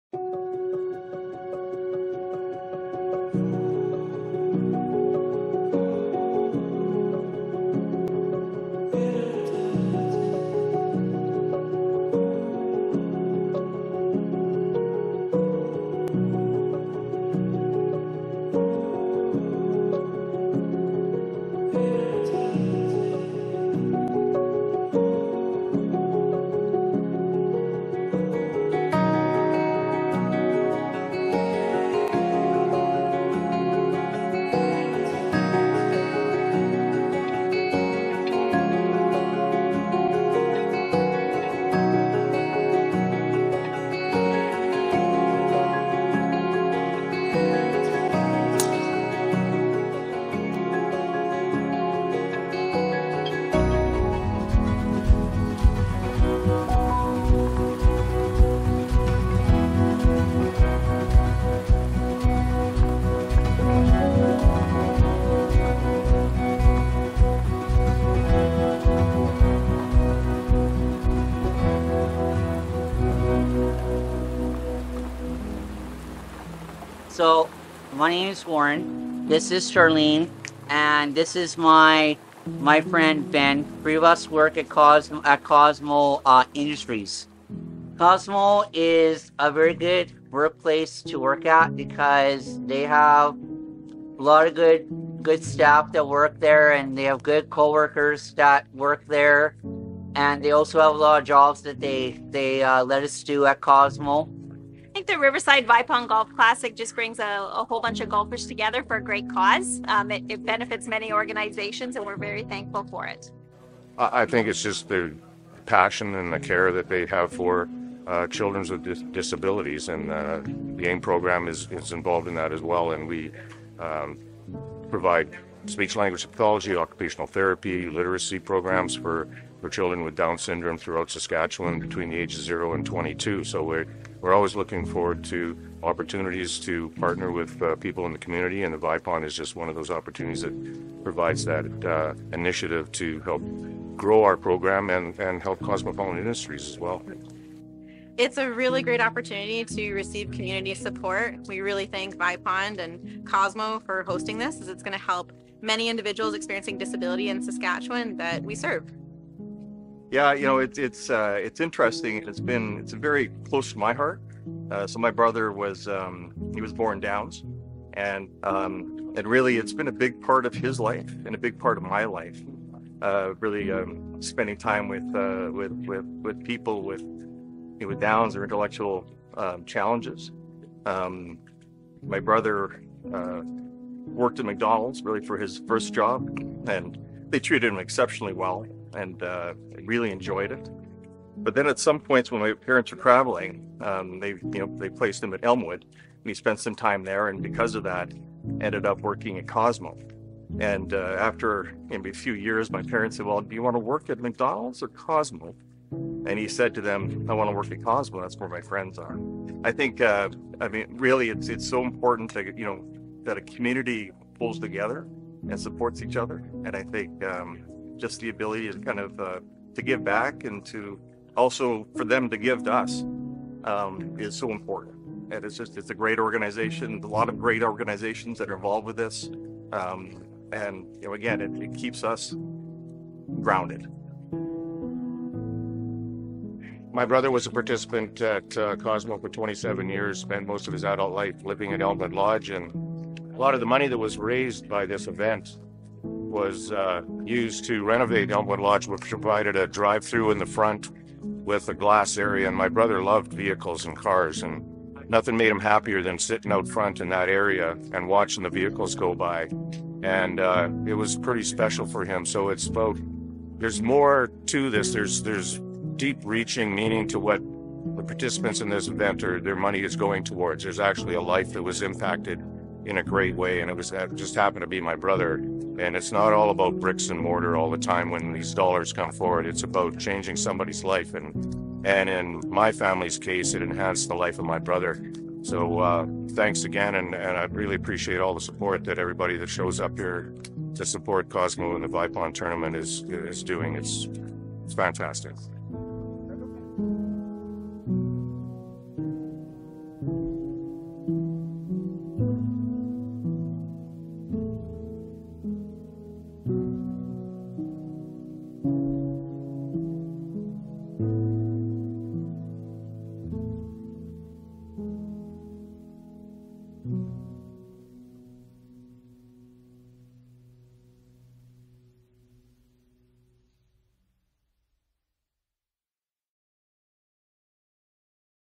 What is the Riverside Vipond Golf Classic?
A golf event held for the benefit of persons with intellectual and related disabilities, held at Riverside CC.  This year the tournament will be on Monday June 19th, 2023.  This short video, shot during last year's event, showcases what the Riverside Vipond is all about.  It includes interviews with those who benefit from the funded programs and the organizations who offer those programs.